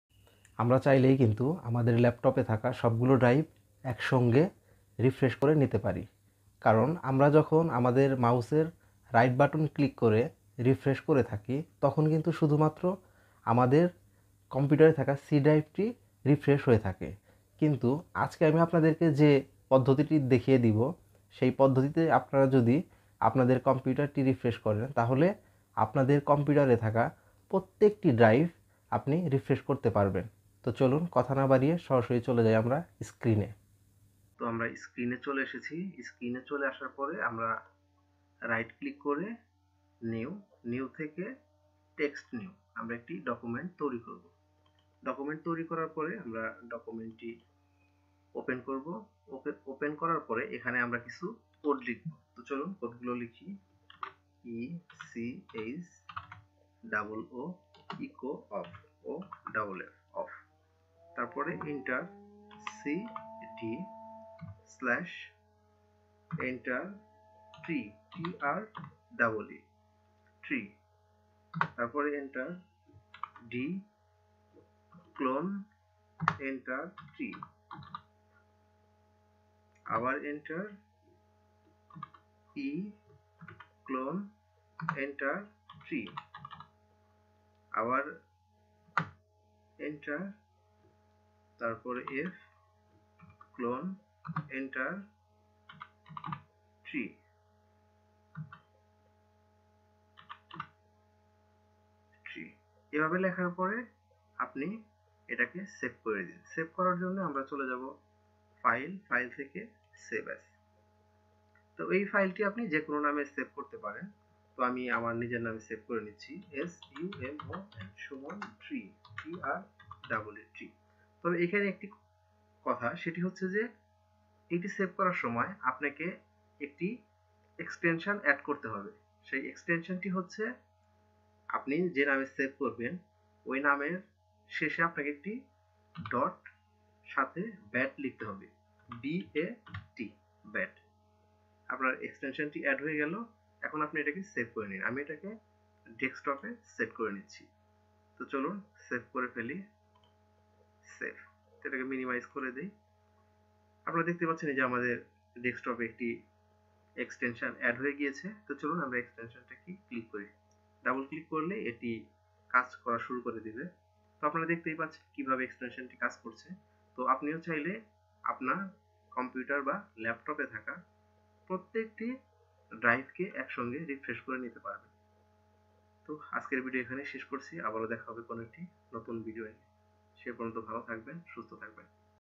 How To Auto refresh on your computer Bangla
How To Auto refresh on your computer Bangla tutorial, Just One click any driver refresh running, very simple tutorial, just follow this video, than fast working your computer